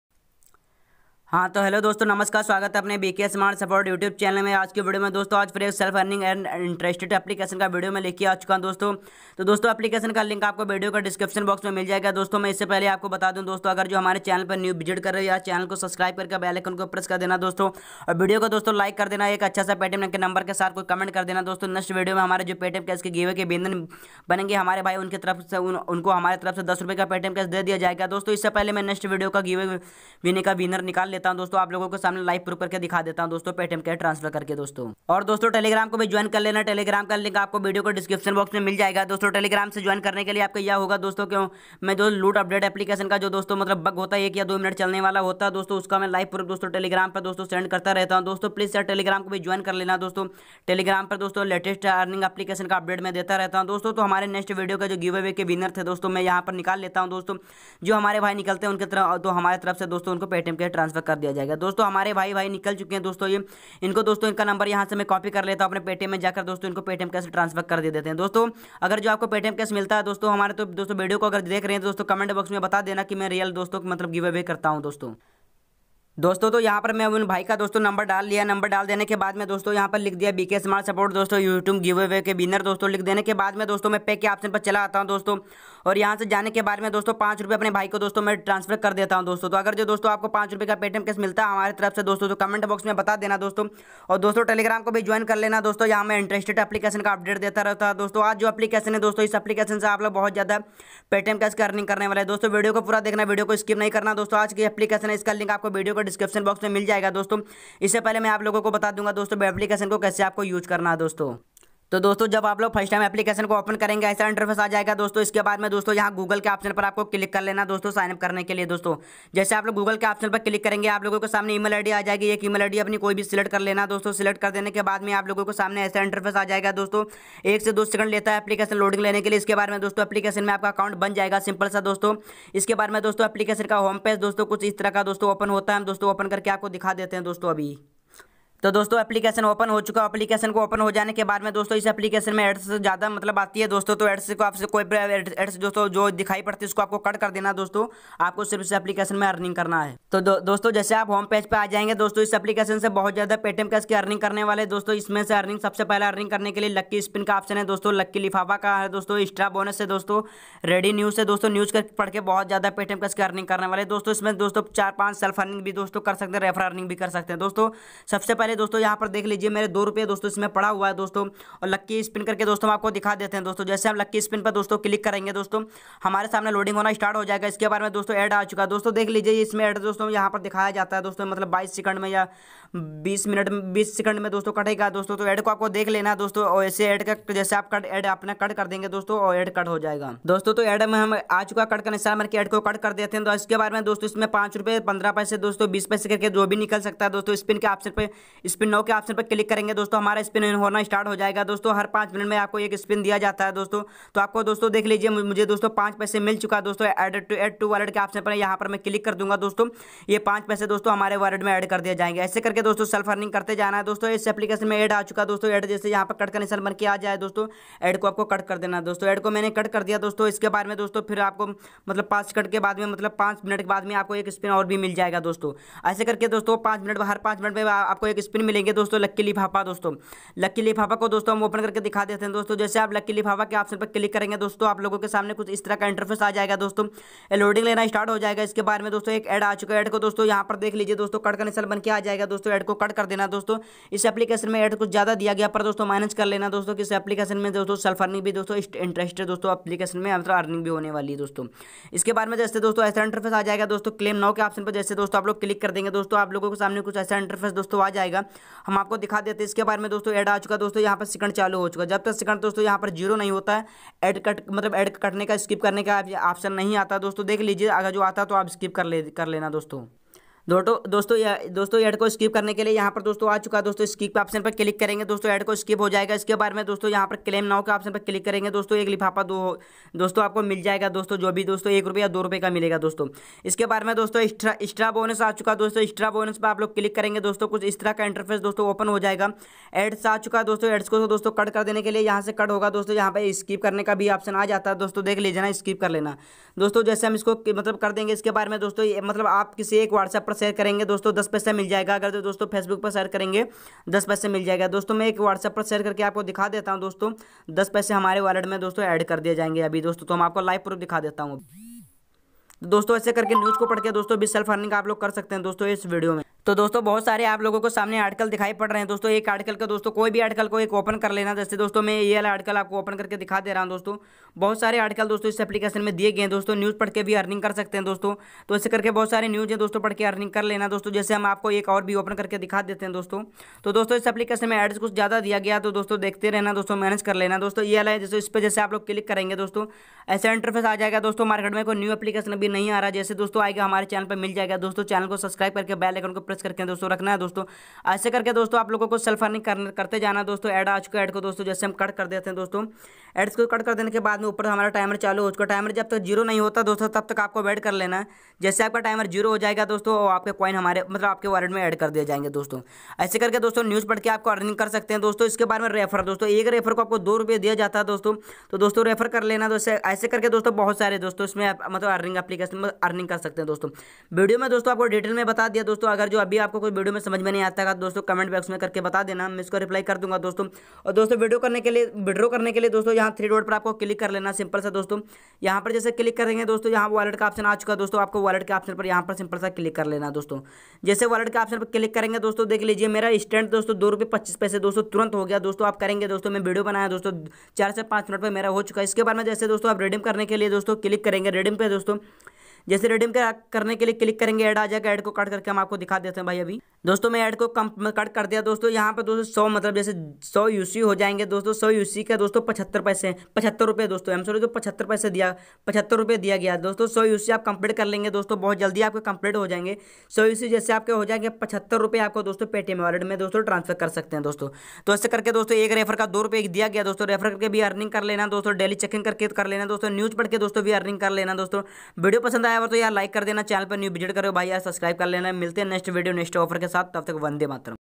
₹10+₹10+₹10 Paytm Cash तुरंत मिलेगा New best Earning App !! Instant Redeem paytm wallet
Refer code➡️1211

Doston is video mein aap log video ko dekh kar self earning Paytm cash ki earning unlimited kar sakte ho refer and self earning kar sakte ho new interested earning app hai

Paytm new earning app
Paytm new earning app unlimited trick
Paytm new loot update
Spin and earn app
App unlimited trick